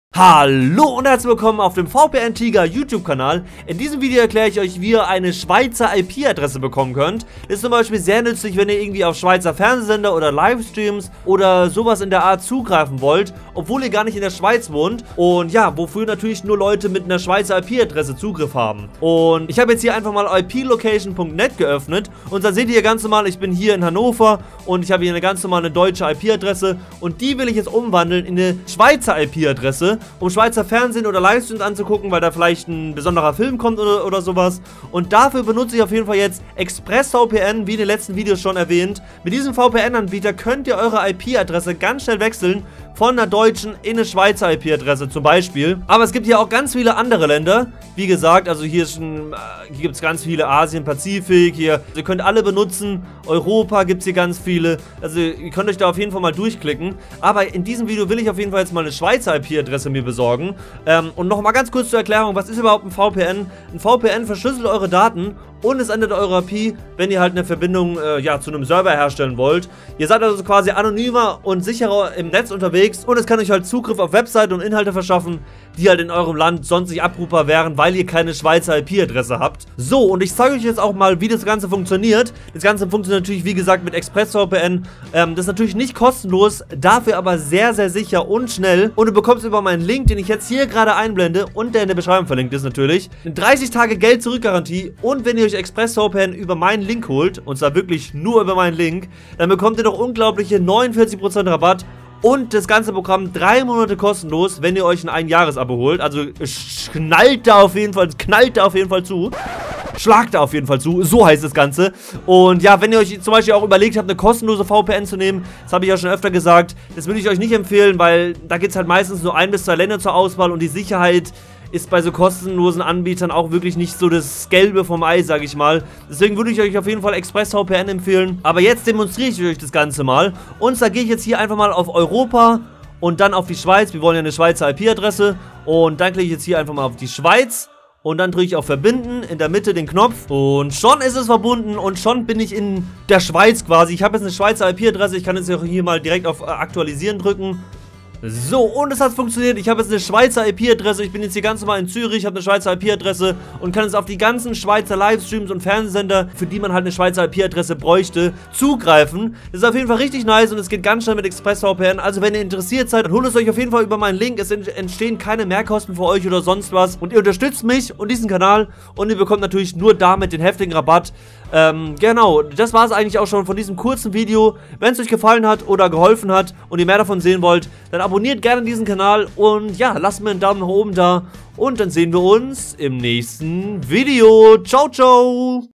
SCHWEIZER IP-ADRESSE: Wie man eine IP-Adresse aus der SCHWEIZ bekommt!
Du willst eine Schweizer IP-Adresse, obwohl du nicht in der Schweiz, sondern in Deutschland wohnst? Ich zeige dir in diesem Video wie du eine Schweizer IP Adresse von überall aus der Welt bekommst! Mit ExpressVPN 🔥

Es ist total einfach und ExpressVPN funktioniert mit nur mit wenigen Klicks:
2.) Installier' dir die App und starte sie
3.) Wähle einen Schweizer Server aus
4.) Klick auf den Knopf in der Mitte zum Verbinden
Und schon hast du eine Schweizer IP-Adresse und kannst auf Inhalte zugreifen (Schweizer Fernsehsender etc.), die in deinem Land normalerweise nicht verfügbar sind! Viel Spaß!

🔔 Aktiviere die Glocke, damit du kein zukünftiges Video mehr von mir verpasst!
✏️ Schreib deine Meinung zu diesem Video gerne in die Kommentare! ☺